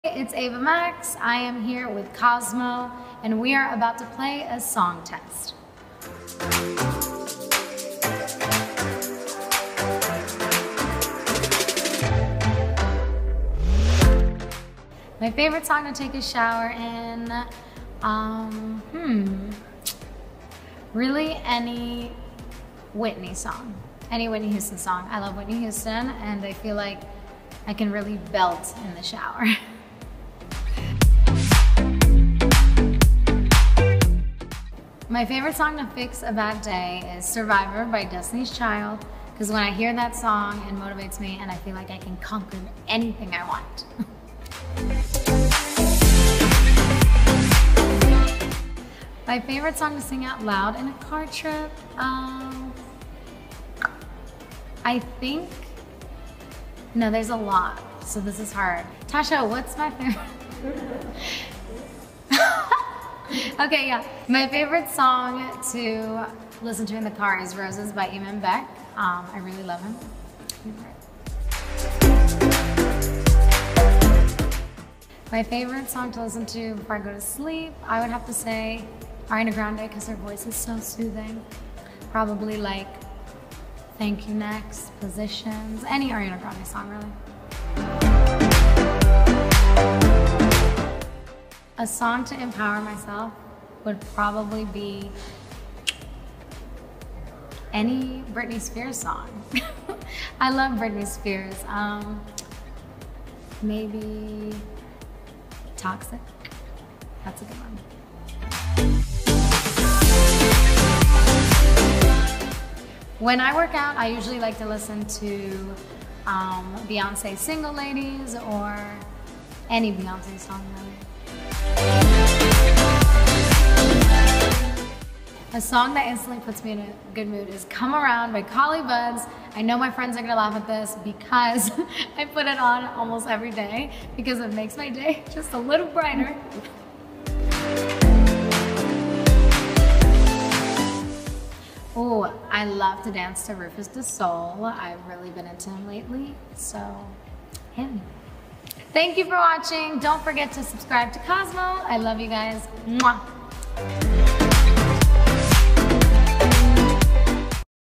‘El test de las canciones’ con Ava Max - Cosmopolitan España
La cantante también tiene un tema favorito para cantar en la ducha o uno para empoderarse cuando no se siente bien.